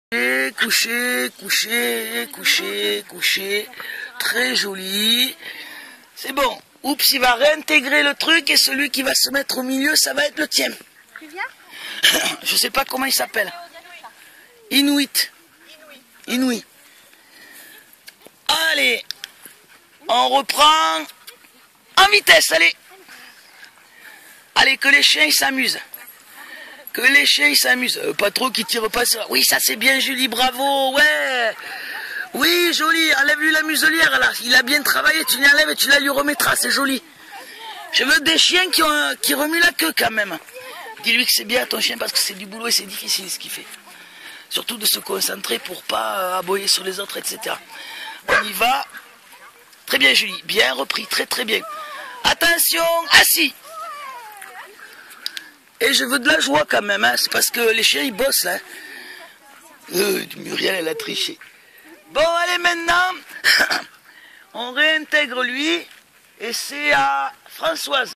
VDMp, une équipe au service de nos chiens : le cours du jour Dabsi, Ghost Inoui...
Dabsi travail en muselière (pour dédramatiser, parce qu'on va en avoir besoin)
Les autres, stabilité et gestion des aboiements pour  Ghost et Inoui.
Le travail par le jeu ;)
À VDMp, nous apprenons à comprendre nos chiens en positif.